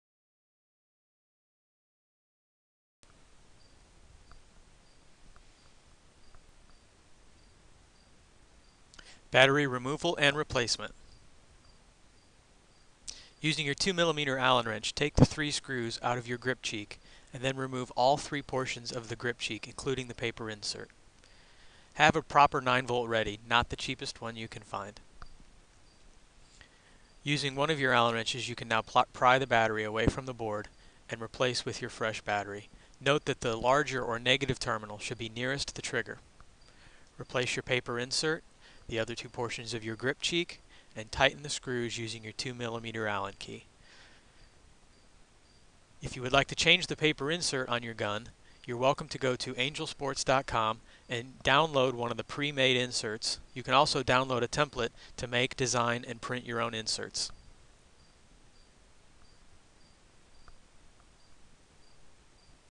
Angel Sports Tech Video - Battery Removal and Replacement - Angel AR:K/SB
A short technical video describing how to change the Battery on the Angel AR:K/SB Paintball Marker.